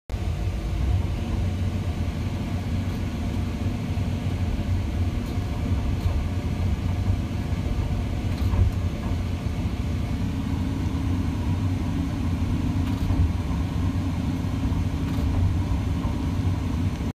2012 CATERPILLAR 349EL For Sale
3,409 IDLE HOURS, SETUP TO RUN A SHEAR, MEDIUM PRESSURE HYD CIRCUIT, HIGH PRESSURE HYD CIRCUIT, QC, NO ATTACHMENTS, VARIABLE GAUGE. LOCATION/CONDITION/HOURS SUBJECT TO CHANGE.